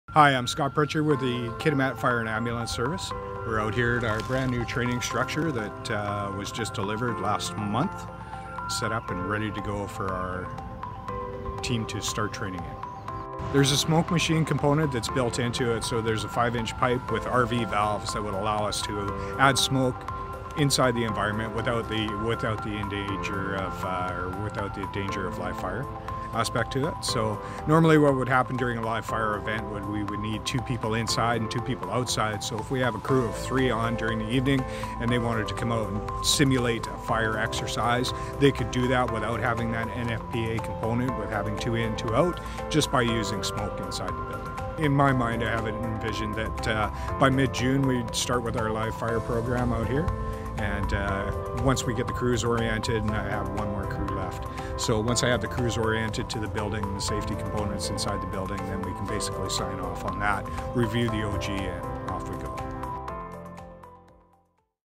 Kitimat's new fire training facility
A new live fire training facility will mean access to a safe learning environment for Kitimat's fire fighters.